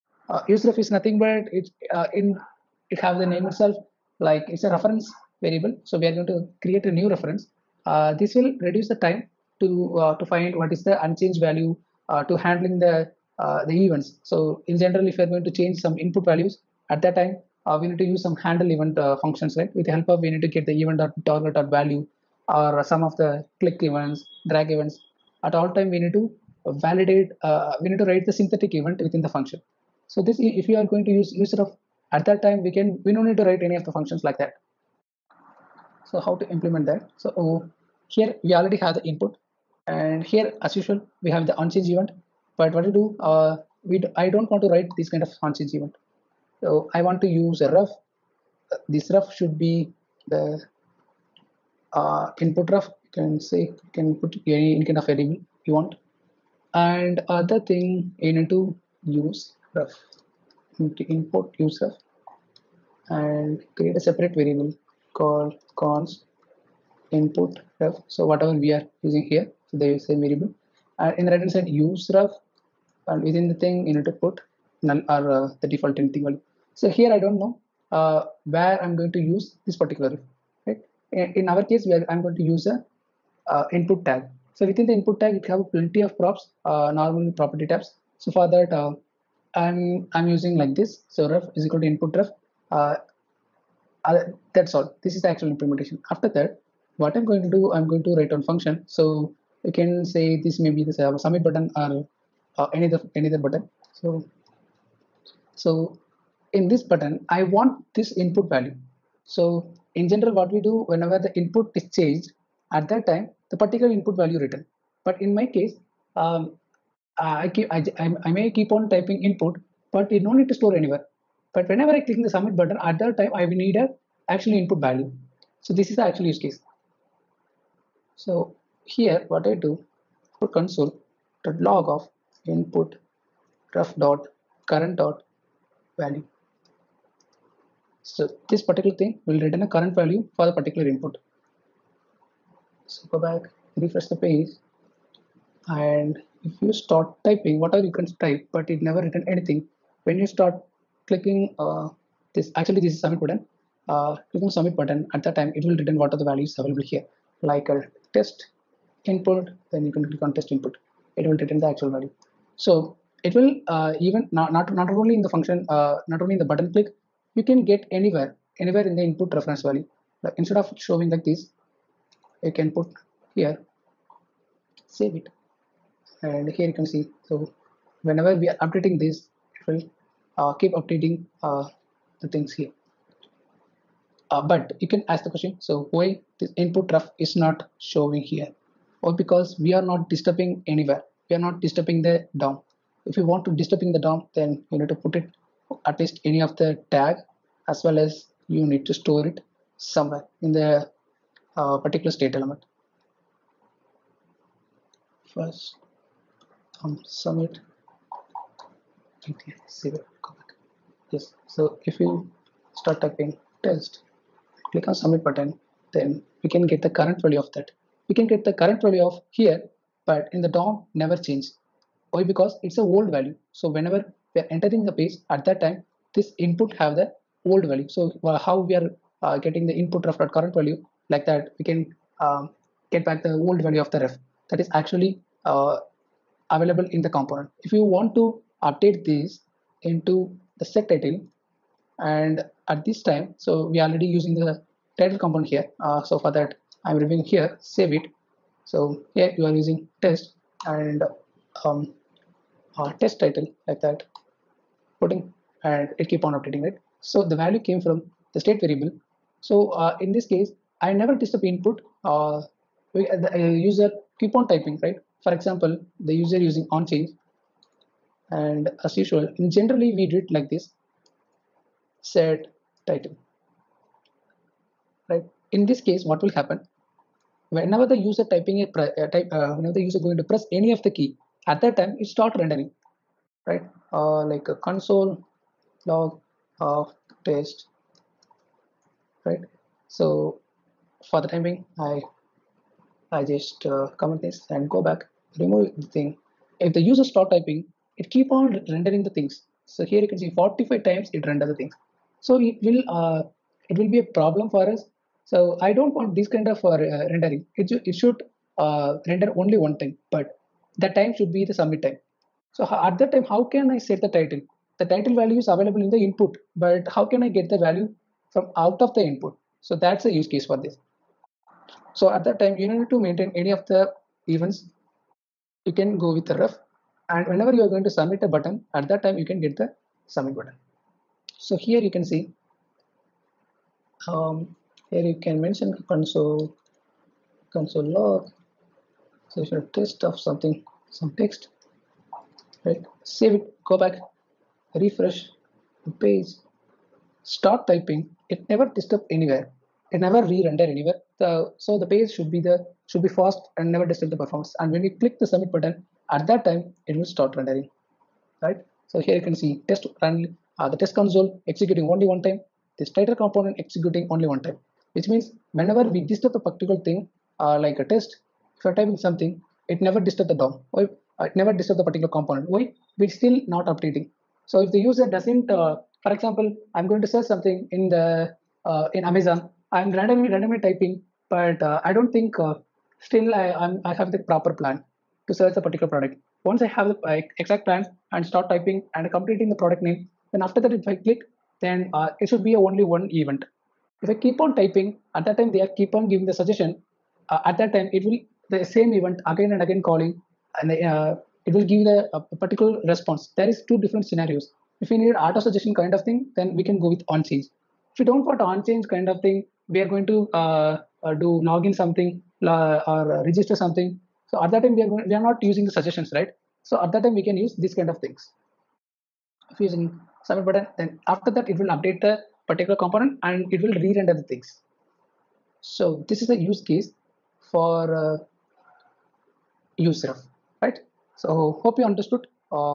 useRef | React Js
Covered Topics,
- What is useRef?
- In which scenario, need to use useRef?
- How to implement useRef with input/form?